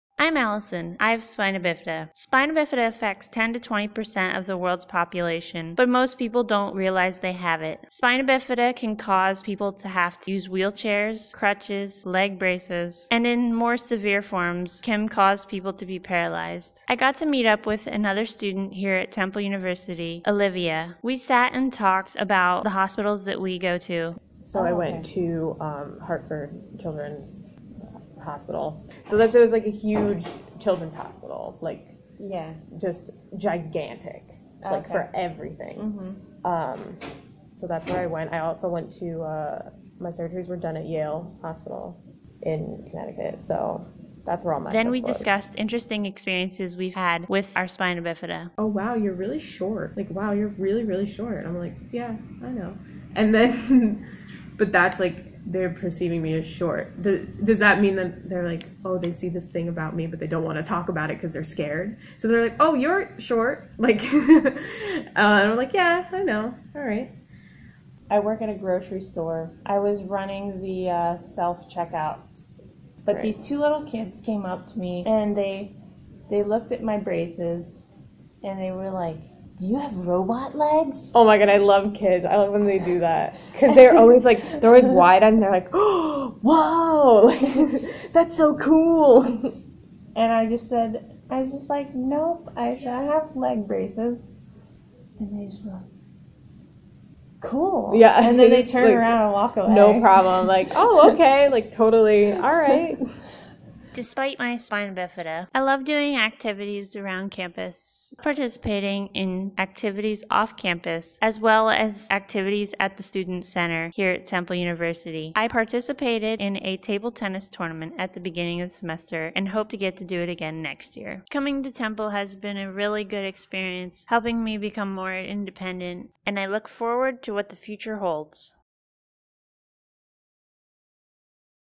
A Glimpse into the life Spina Bifida
Final Revised video for media tech class at Temple University